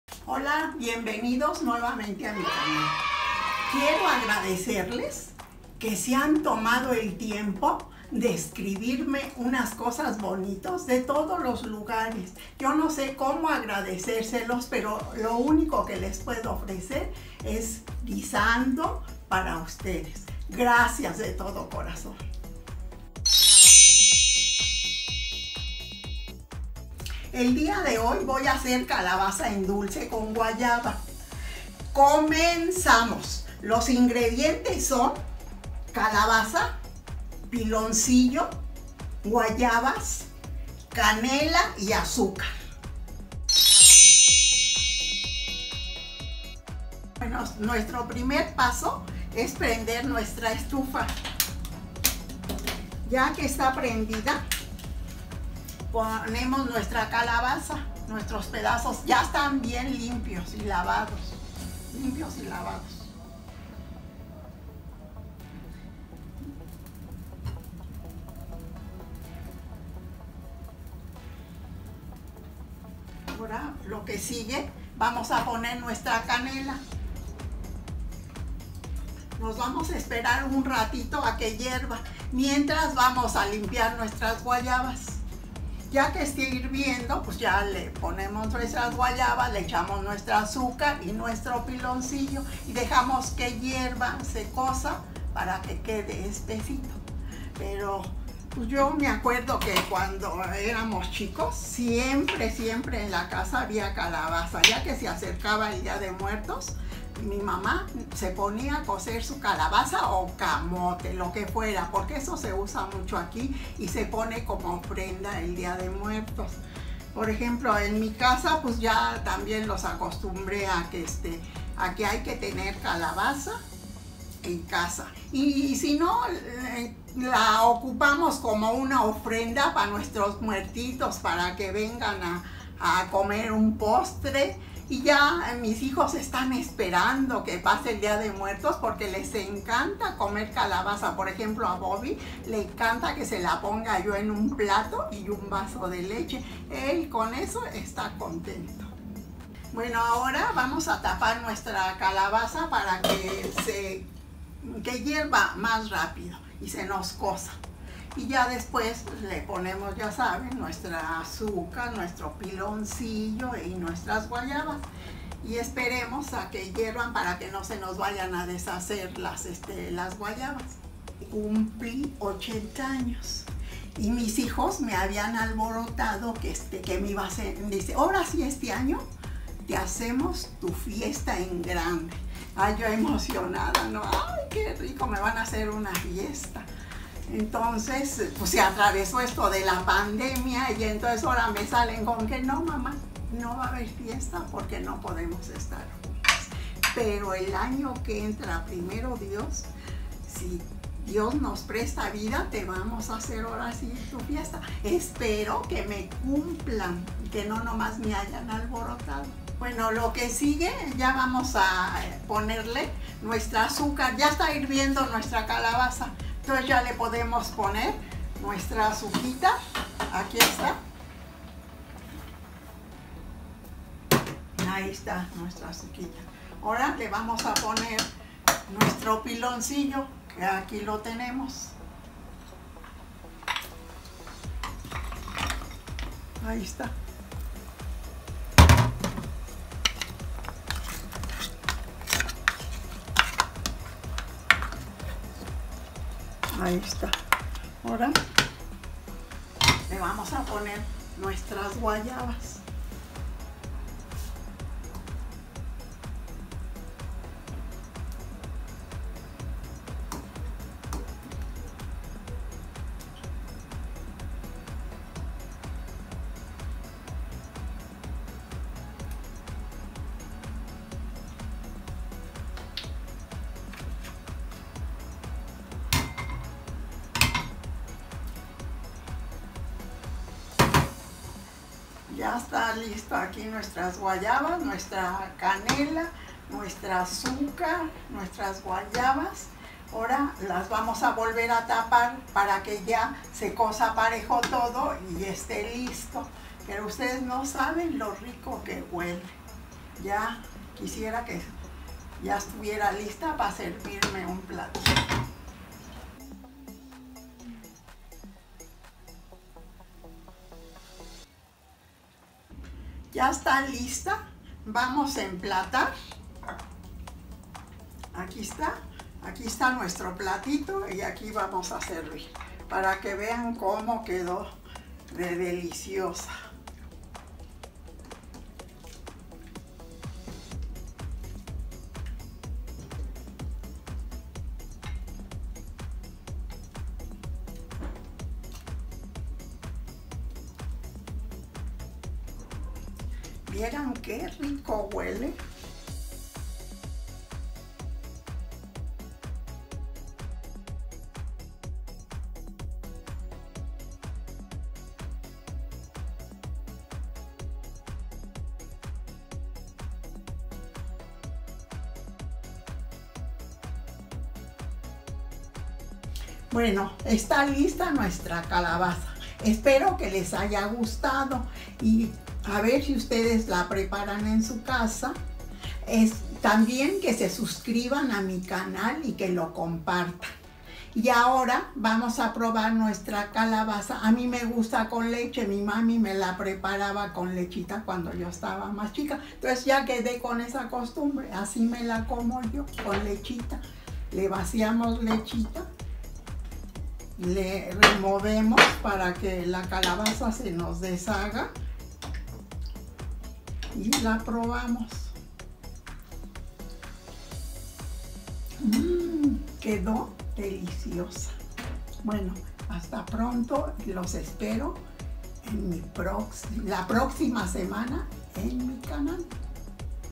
CALABAZA EN DULCE!!!! PARA ESTE DÍA DE MUERTOS...ESPERO LA DISFRUTEN (MAMÁ CHUY)
Nueva receta!!, no se pierdan la receta de calabaza, para el día de muertos, es tan rica, que es una de mis recetas favoritas!!!